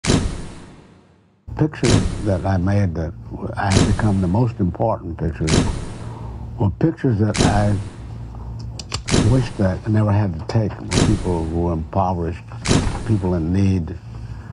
Street Photography And More By Gordon Parks
In this we look at some of Gordon Parks most poignant images as Gordon tells us that he really didn't want to take them.